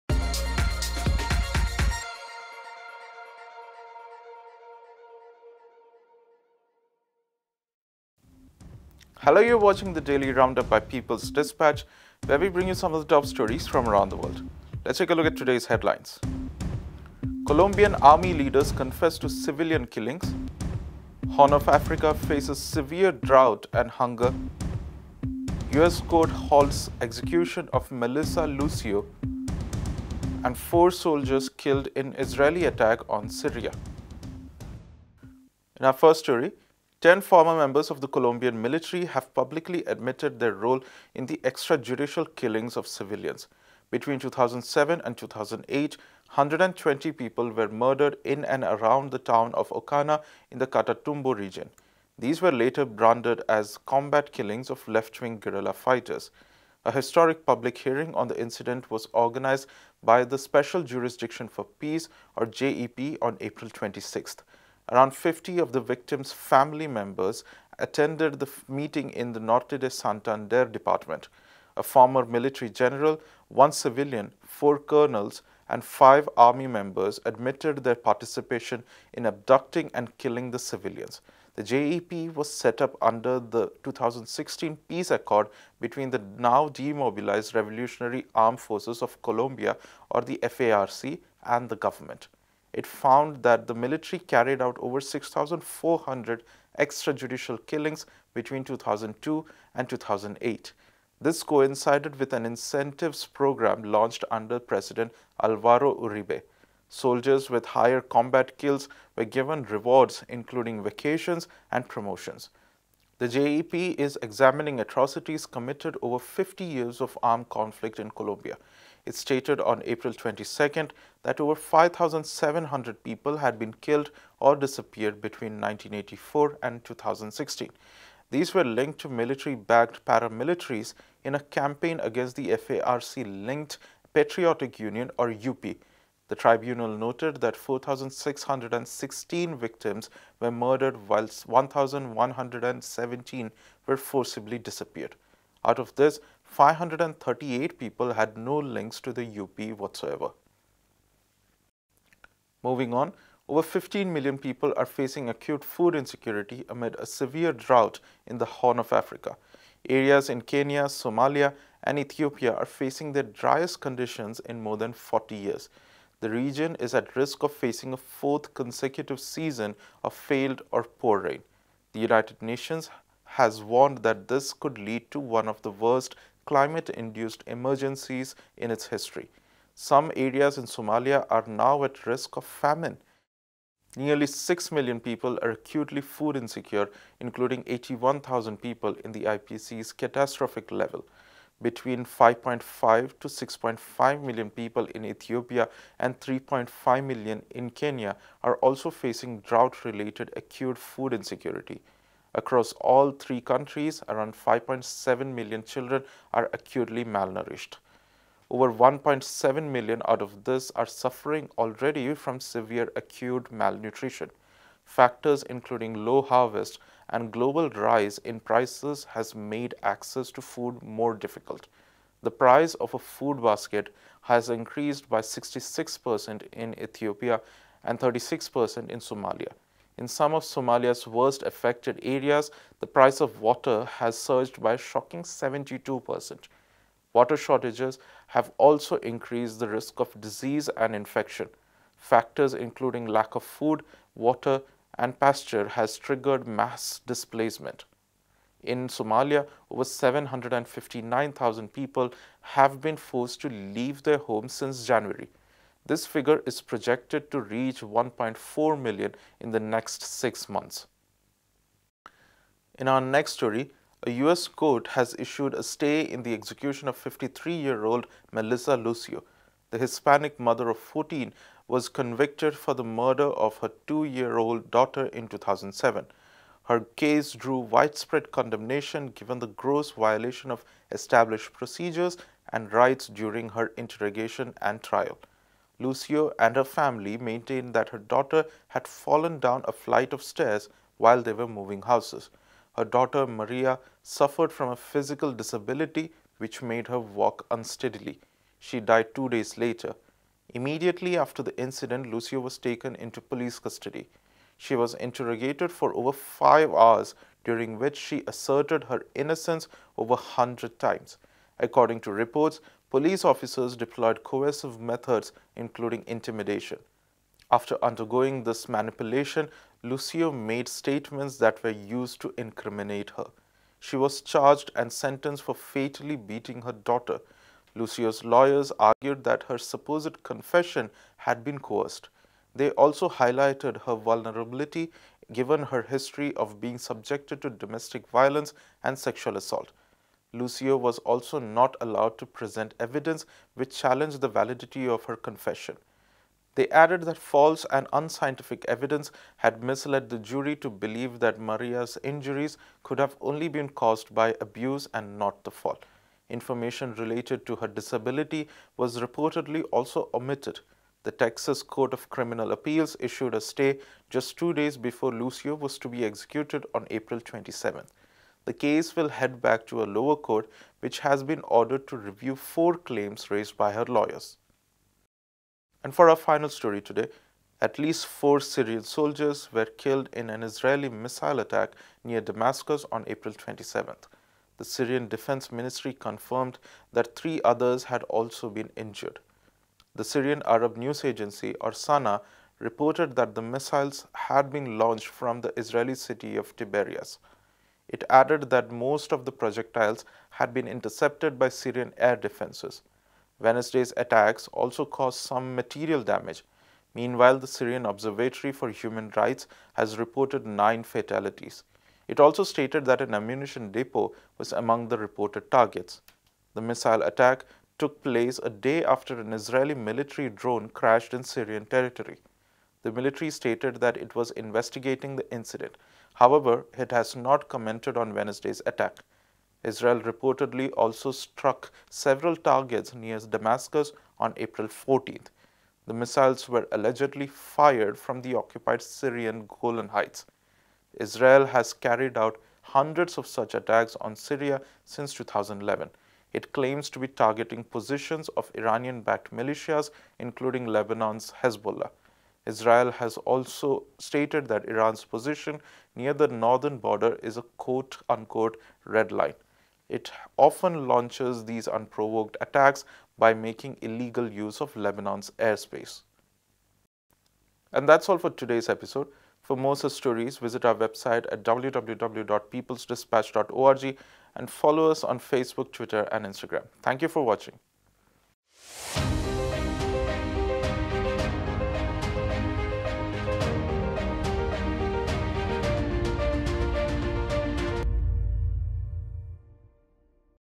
Daily Round-up|Colombian ex-military officers admit role in killing 120 civilians and other stories
In today’s episode of the Daily Round-up we look at a landmark public hearing held by the Special Jurisdiction for Peace on the Colombian military’s role in civilian killings in 2007-08; rising levels of acute food insecurity and mass displacement in the Horn of Africa as the region faces one of its worst droughts; an order issued by the Texas Court of Criminal Appeals to halt the execution of 53 year old Melissa Lucio who was convicted of killing her daughter in a widely-condemned trial; and the killing of at least 4 Syrian soldiers in an Israeli missile attack near the capital of Damascus.